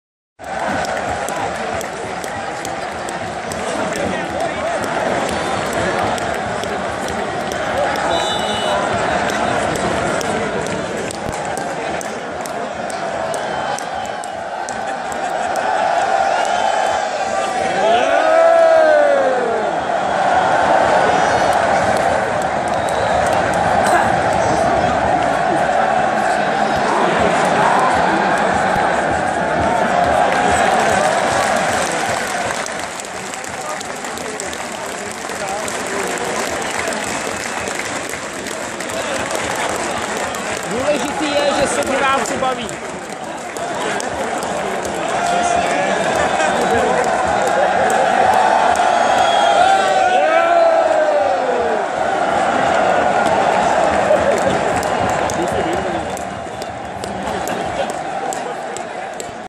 Mexicka vlna zapas MOL Cup 2016 - Opava-Plzen 4:2
Divaci v Opave se bavi na vyprodanem stadionu. Jako plzensky fanda musim rict jen jedine. Vykoupano, jede se domu. Atmosfera uzasna, zadne zakerne fauly, hezky fotbal.